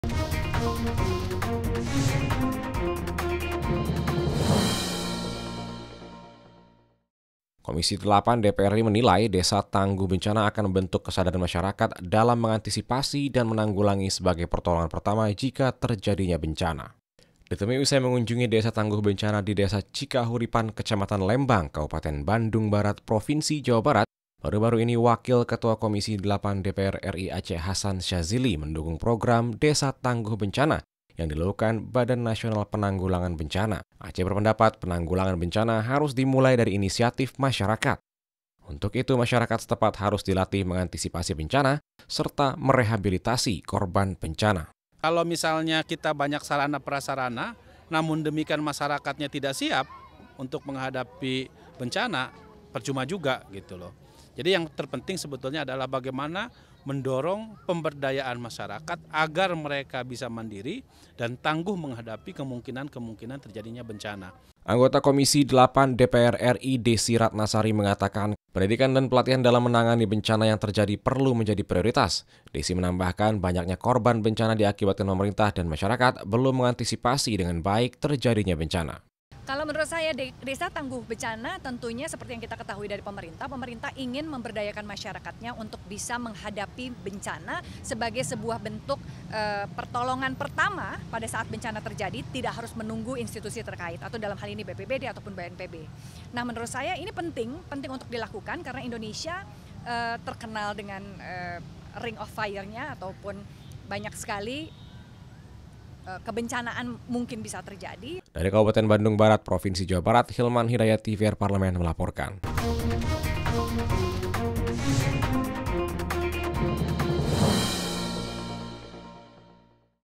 DPR RI - KOMISI VIII DPR RI DUKUNG PROGRAM DESA TANGGUNG BENCANA
NARSUM : TB. ACE HASAN SYADZILI / Wakil Ketua Komisi VIII DPR RI / FPG / Dapil BANTEN I
DESY RATNASARI / Anggota Komisi VIII DPR RI / FPAN / Dapil JABAR IV